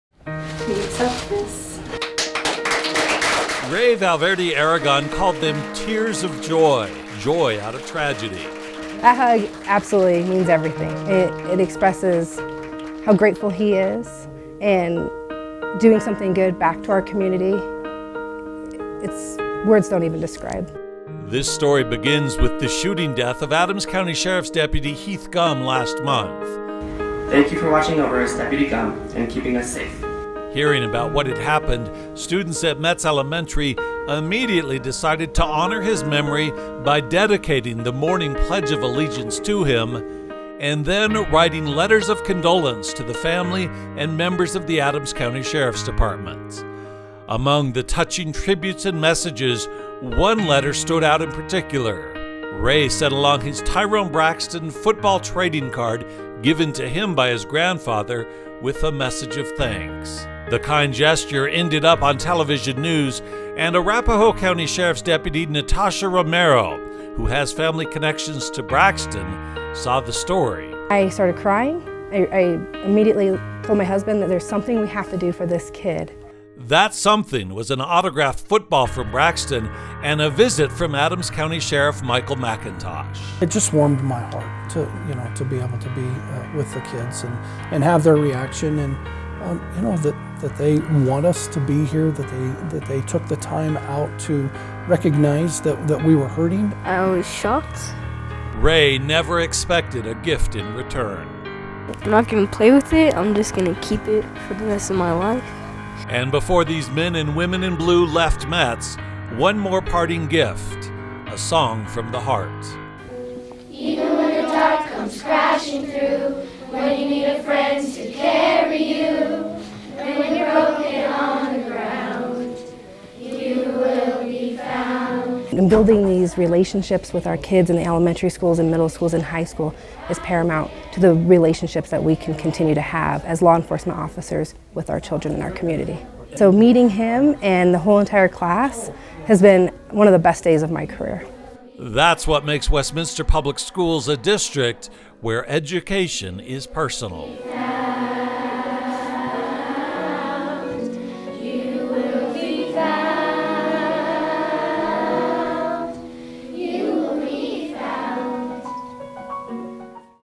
Giving Comes Full Circle at Metz Elementary
When Metz Elementary students wrote letters of support to the Adams County Sheriff’s Department and the family of fallen Deputy Heath Gumm after he was killed on the job, they never imagined how many lives they would touch.